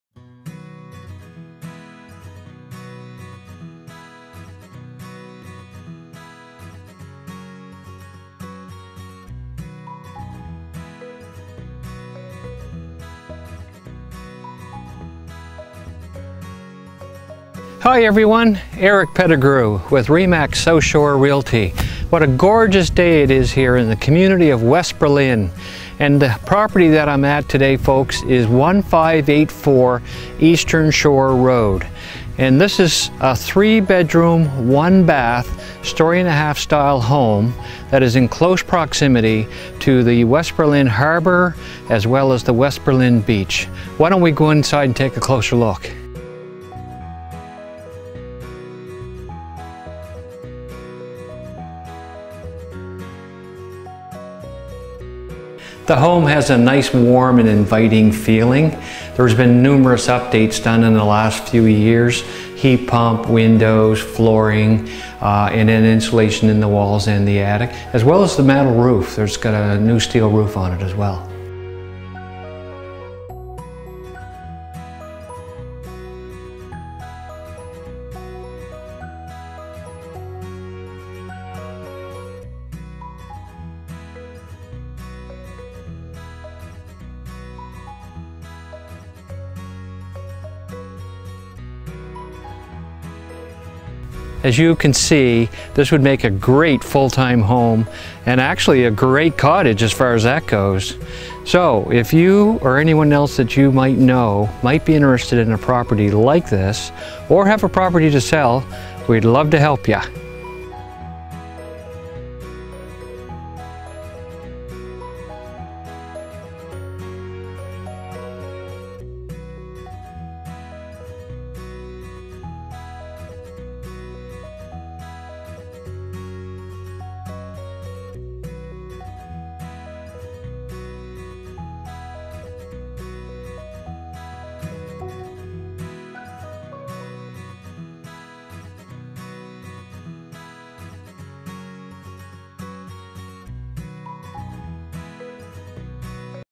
3 Bedroom Home by the Ocean
Great year round home or vacant spot. Home has 3 bedrooms with a possible 4th on the main level. Upgrades over the past few years are windows, insulation, metal roof on house and shed. Heating & Cooling are by way of electric baseboard and a heat pump. Ocean breezes and a view of the harbour add to the charm of this 5 acre property.